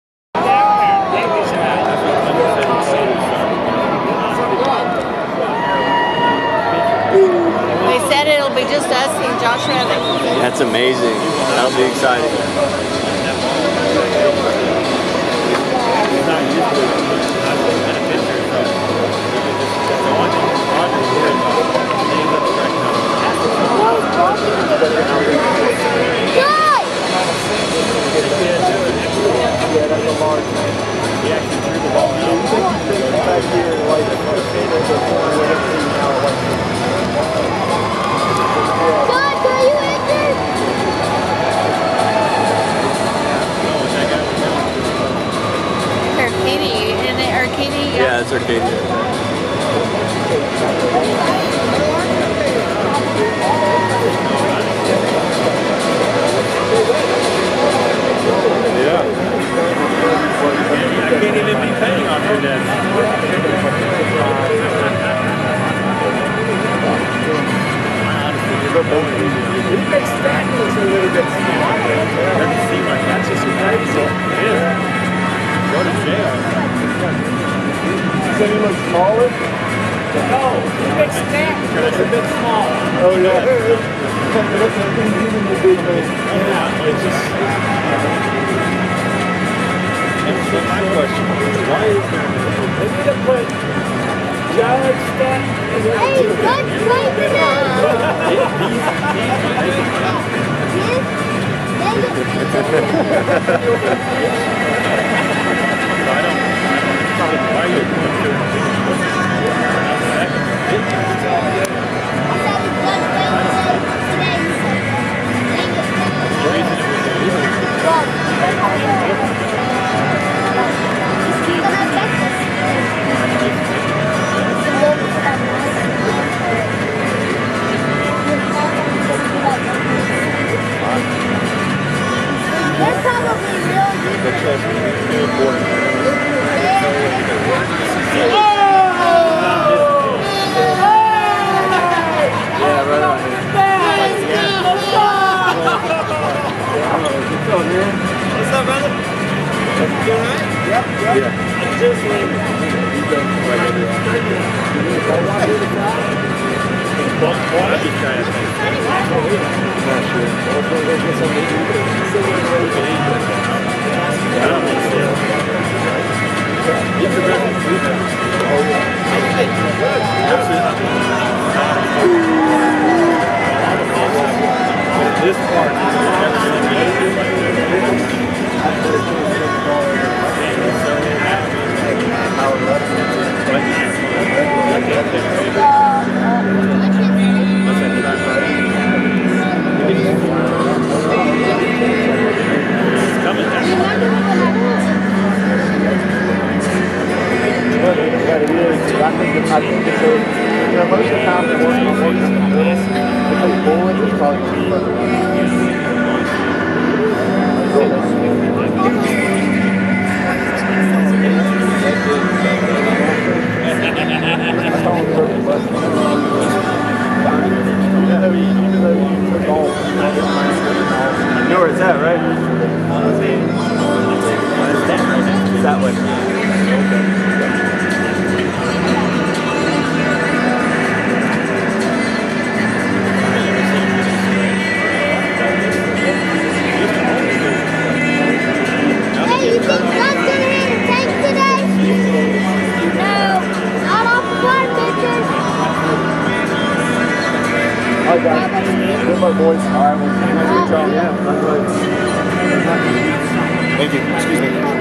Aaron Judge and the Yankees take the field...pre-game warm up...Opening Day @ Houston...3/28/24
The Yankees take the field for the 1st time in 2024 against the Astros in Houston.